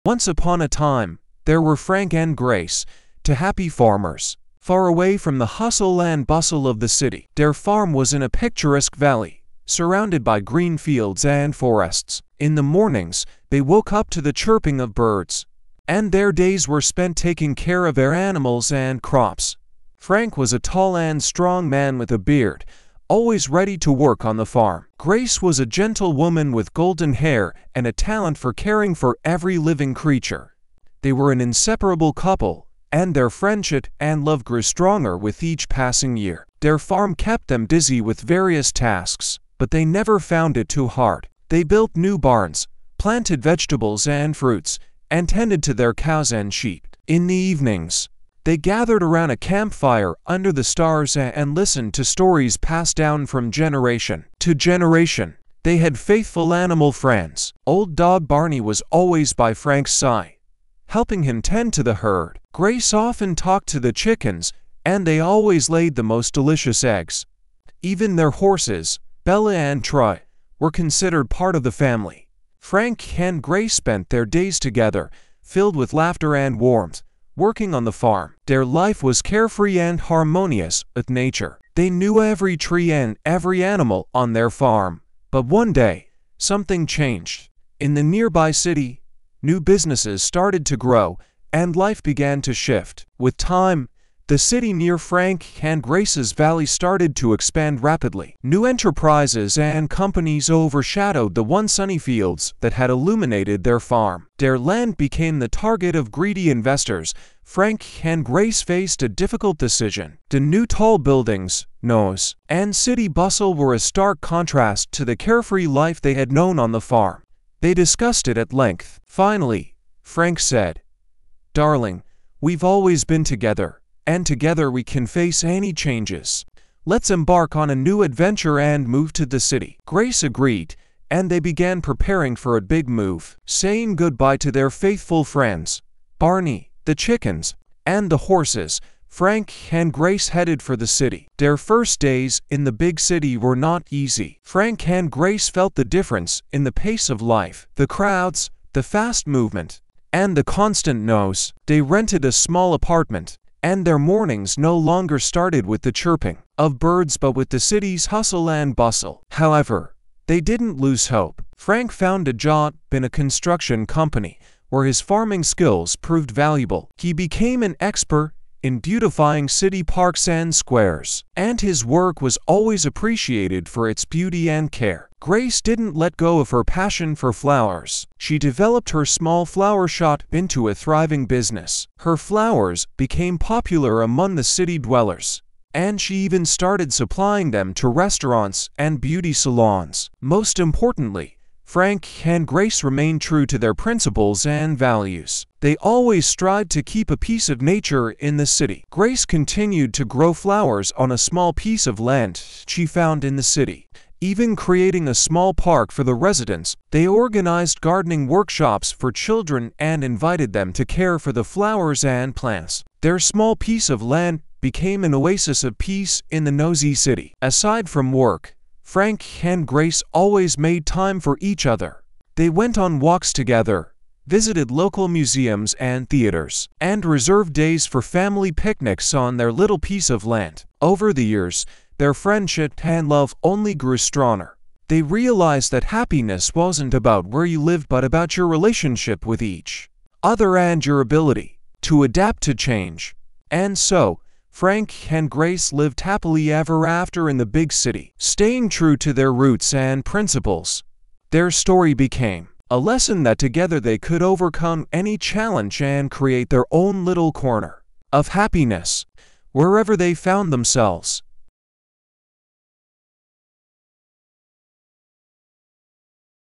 Learn English Through Story Level 1 | Fields to Skyscrapers
Do you want to learn English but find language learning difficult? Join us on this journey through English story level 1, from fields to skyscrapers! Follow along as we learn about some of the most common English words and phrases, and see how they're used in context.

Whether you're a beginner or intermediate student, this story level 1 course is perfect for you! By the end of this course, you'll be able to understand and speak English like a native!